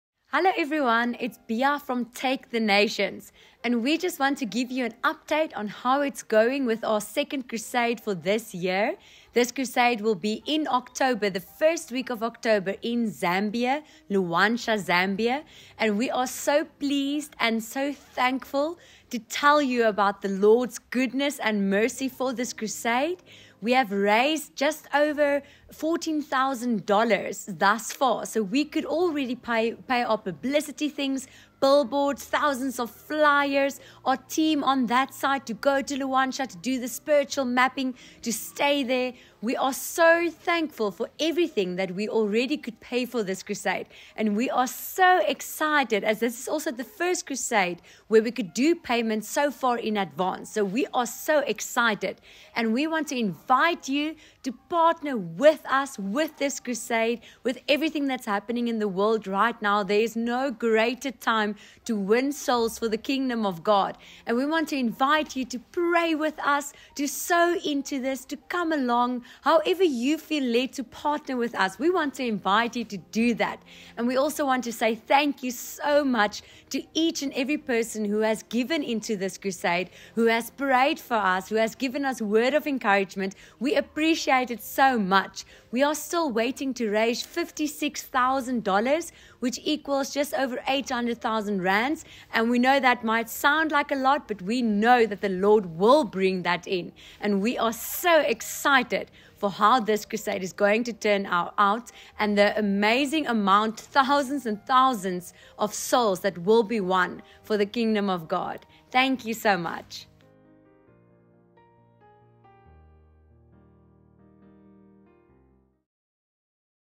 $56,000 TO GO FOR OUR NEXT GOSPEL CRUSADE!
We are so excited to be in a City just outside Luanshya, called Mpatamatu  for our upcoming Gospel Crusade in October. We are expecting to reach over 150,000+ people face to face with the Gospel of Jesus Christ during our 4 day Jesus Crusade, and equip over 300+ Pastors in the City!
We still have a need of $56,100 to cover everything, and we are believing God to see every need met in Jesus Name!
To partner with us on this step of faith, you can click on the link at the bottom.

Daniel & Bea❤️